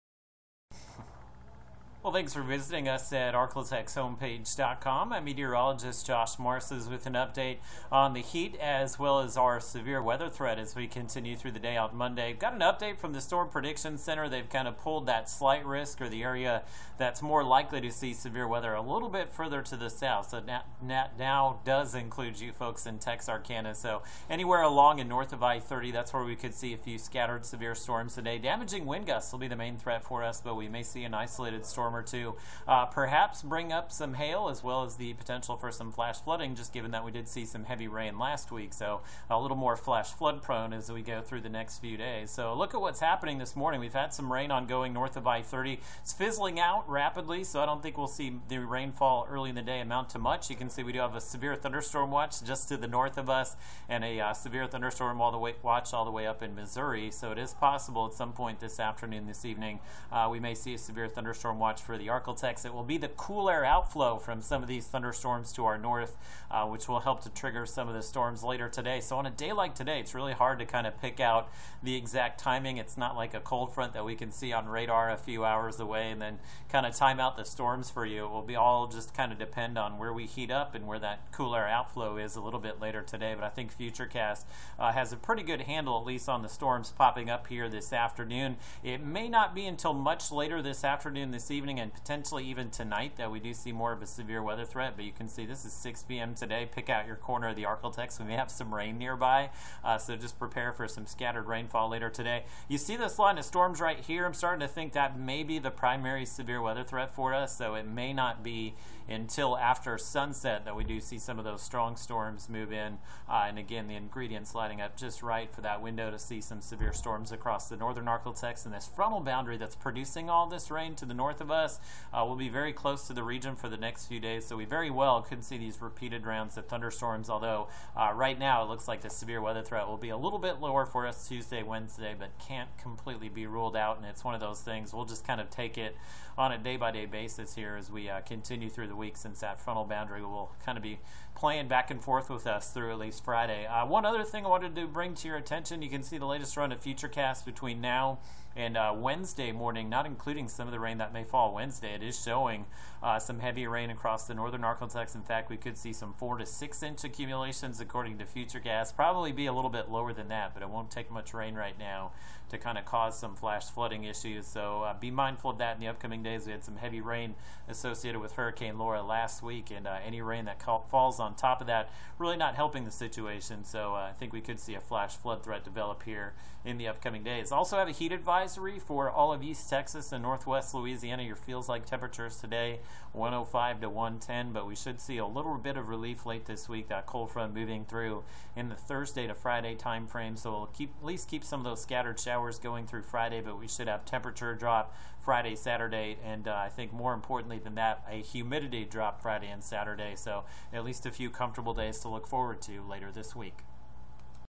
9 a.m. wx update Monday
Updated Monday morning weather webcast for potential severe storms